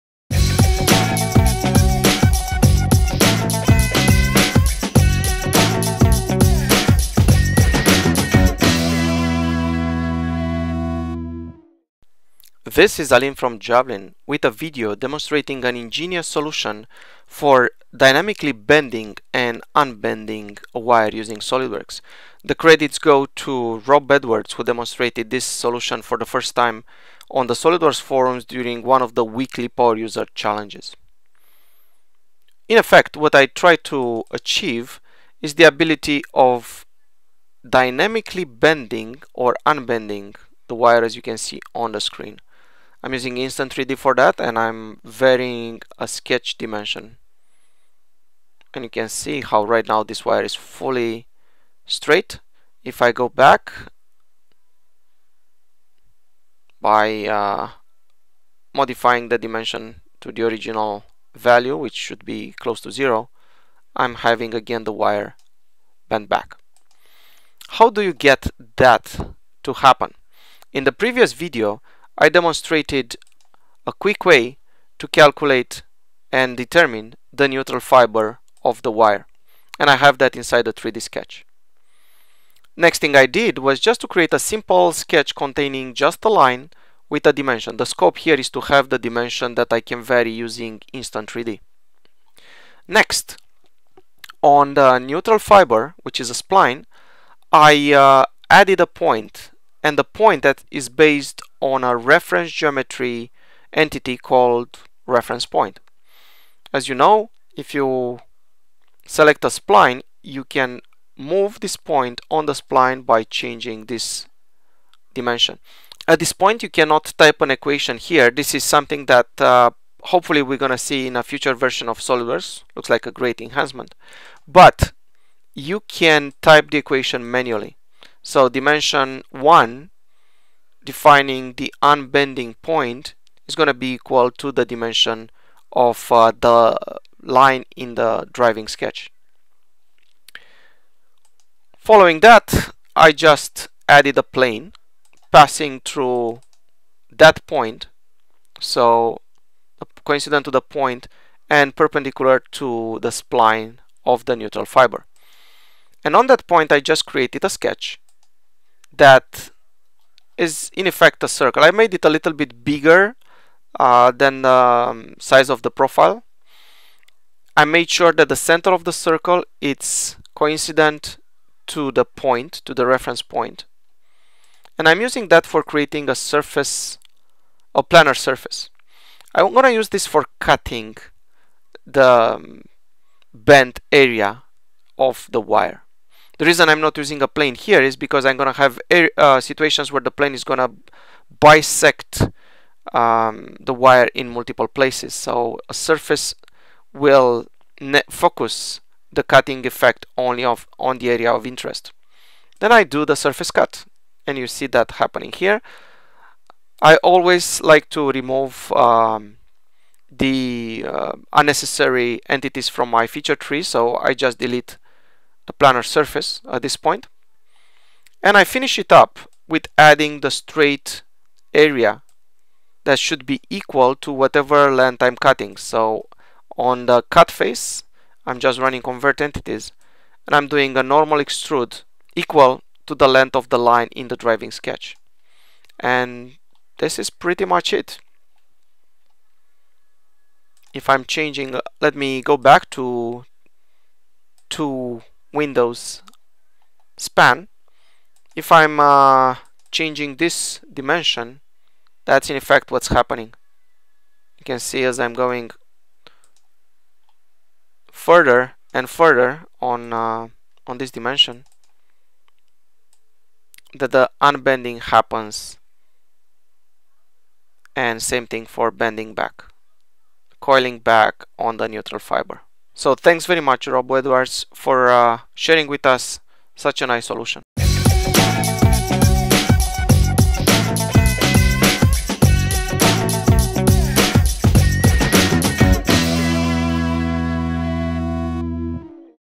SOLIDWORKS Tutorial: Dynamic Bending / Unbending a Wire, Tube or Bar
Rob Edwards, a SOLIDWORKS DEMIGOD, invented a very elegant solution for dynamic bend/unbend wire, tube or bars operations.